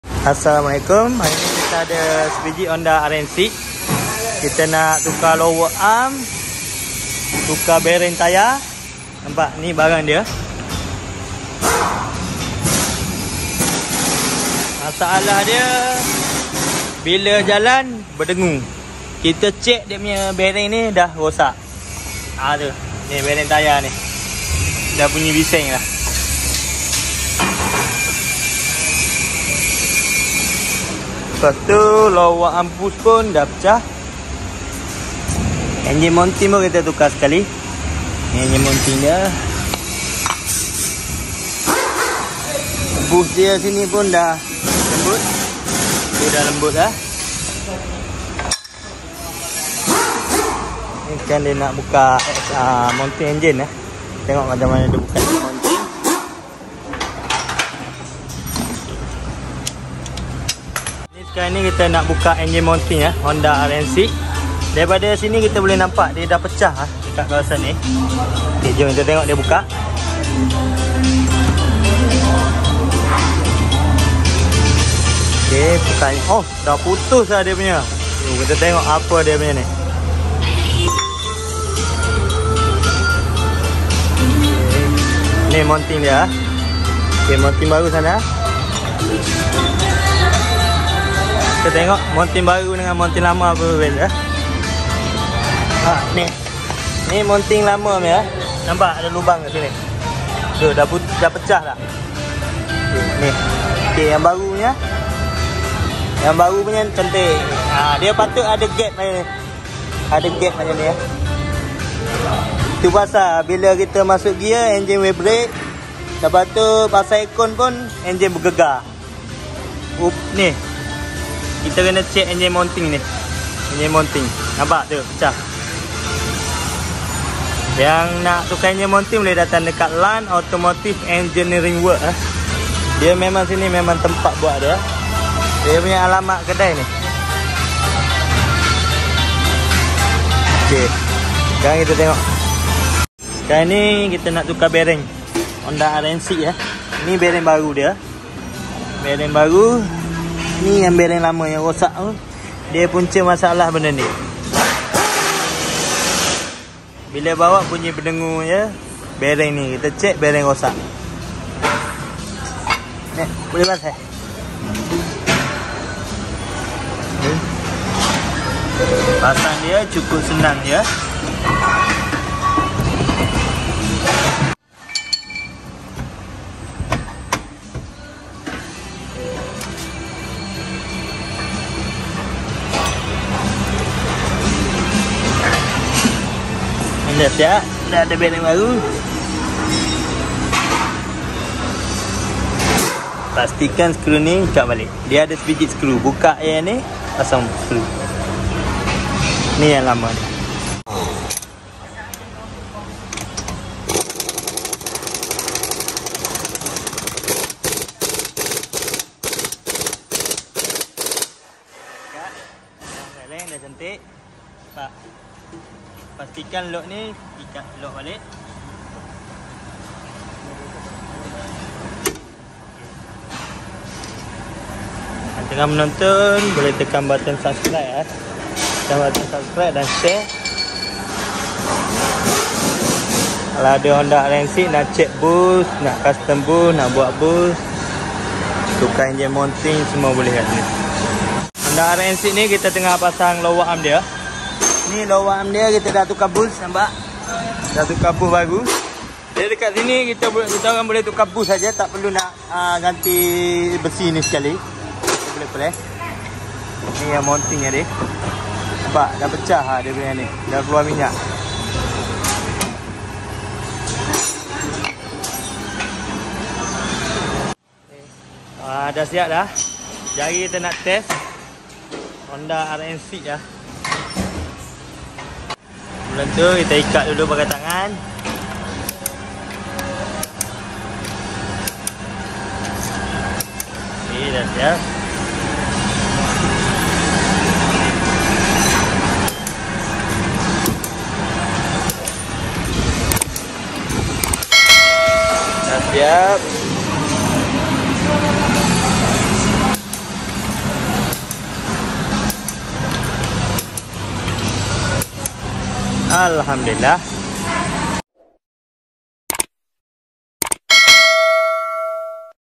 HONDA RN6..SENYAP TERUK❗️❗️ TUKAR BEARING TAYAR,LOWER ARM DEPAN,LINK,LAN AUTOMOTIVE ENGINEERING🙋‍♂️
LOT 1248 JALAN KPAT 3B KAMPUNG BARU AMPANG TAMBAHAN

waktu operasi
9pagi sampi 7malam